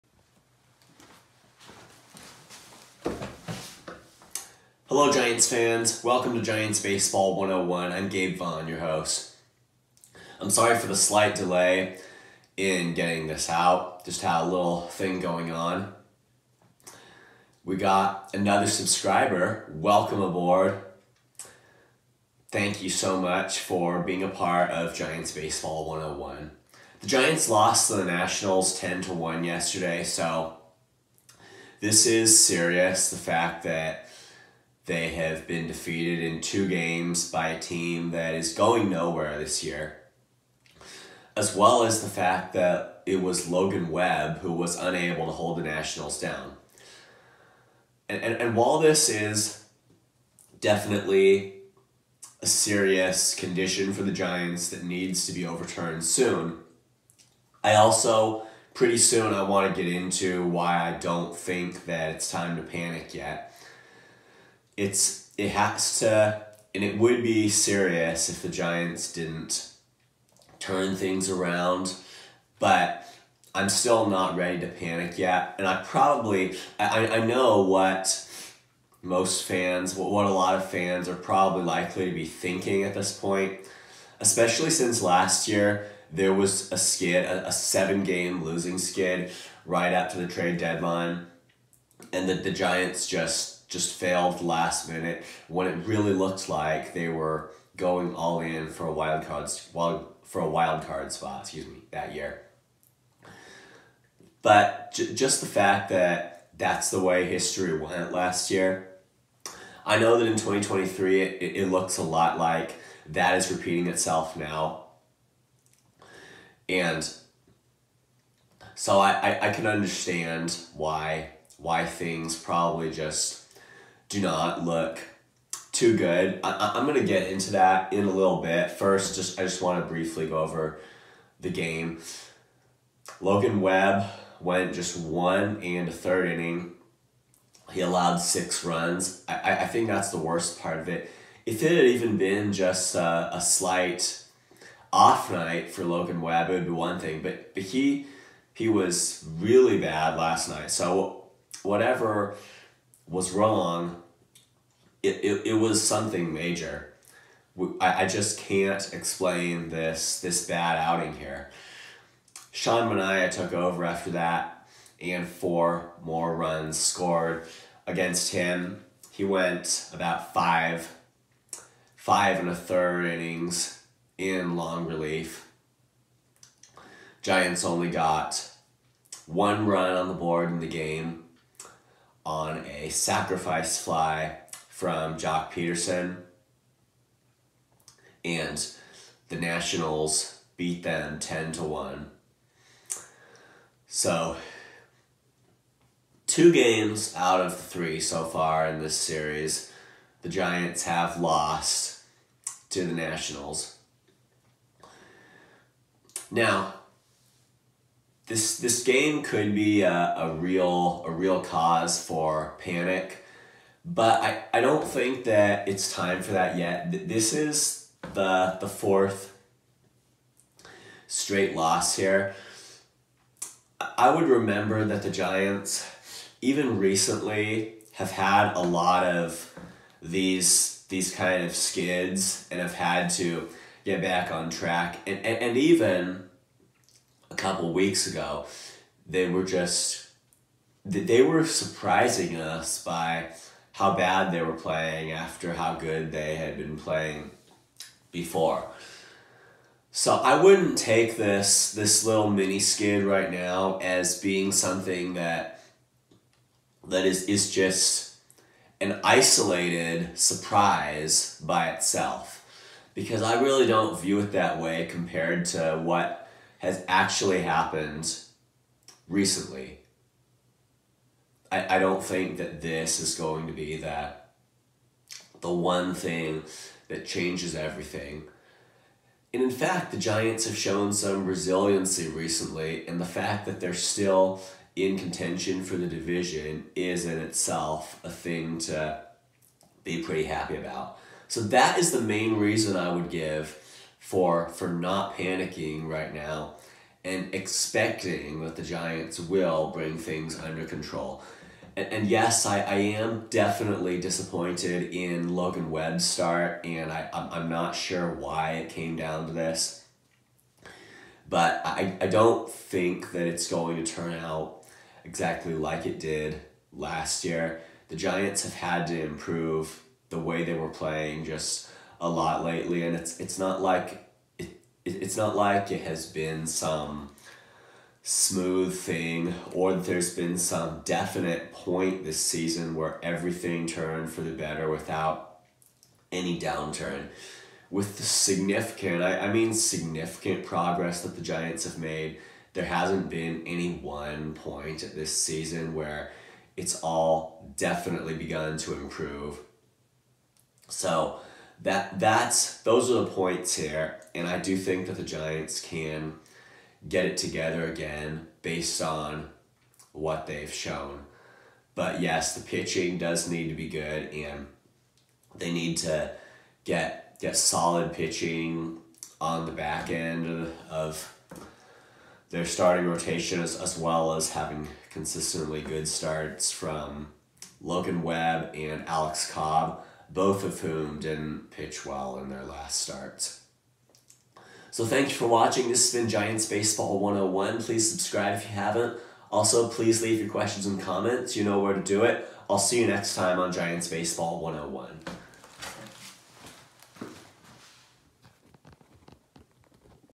Giants Game 07/22/23: Logan Webb Had a Bad Outing, and the Giants Dropped the Series
The Giants were defeated 10-1. (Just making clear that it was J.D. Davis who hit the sacrifice fly. I accidentally said Joc Pederson.)